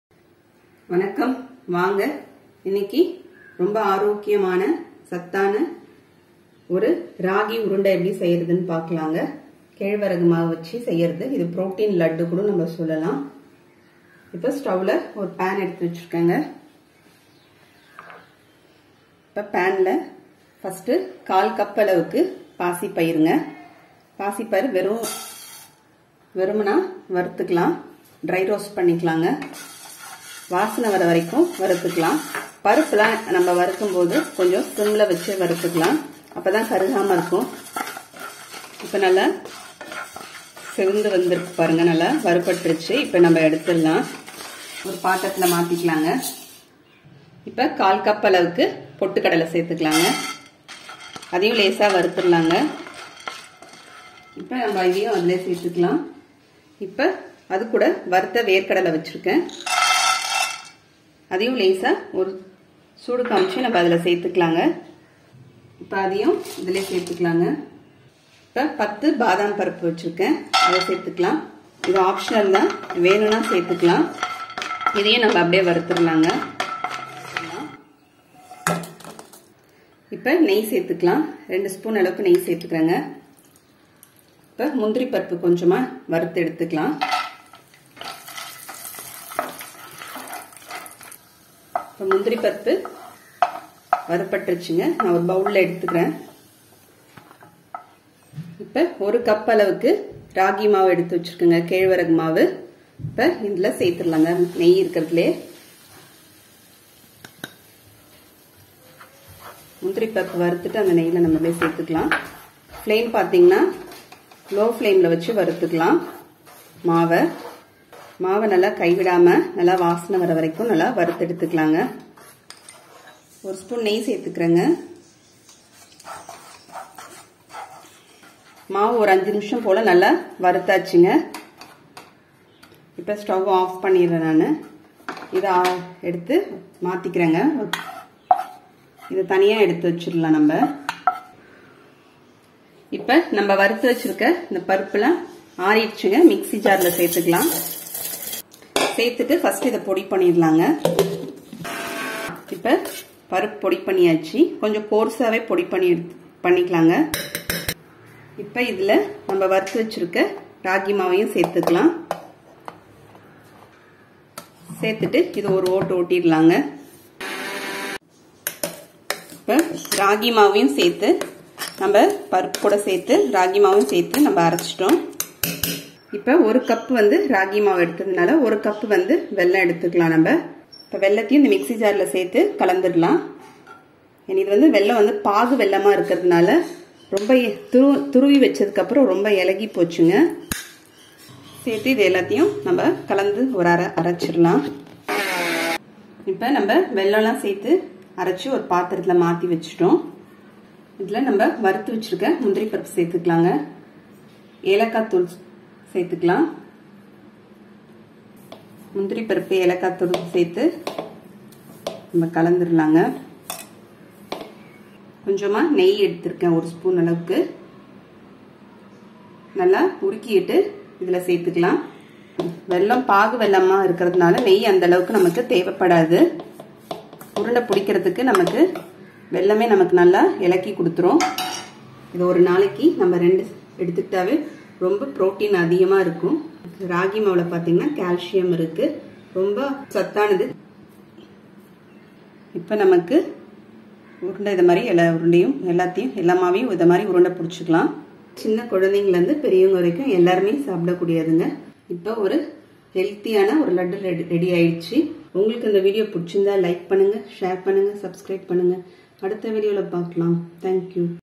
Ragi Ladoo Recipe in Tamil | Kezhvaragu Laddu | Healthy Kids Sweets | Protein laddu
This video is describing about how to make healthy Ragi ladoo recipe in tamil